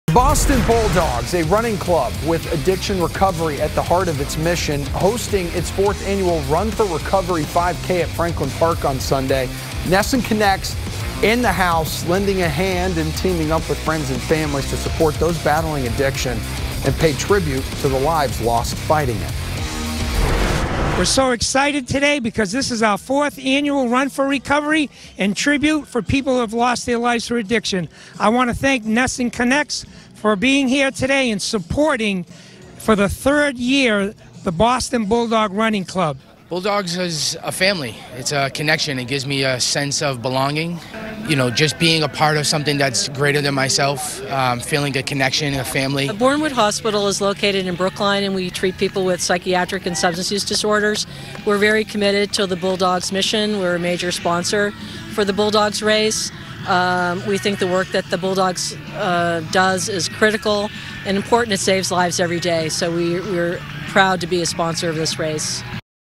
Boston Bulldogs Host 4th Annual "Run For Recovery"
NESN Connects supported the 4th Annual "Run For Recovery" event hosted by the Boston Bulldogs Running Club.  The event is a tribute to those who are battling or have lost their lives to addiction, and the community of family members and friends who support wellness and recovery.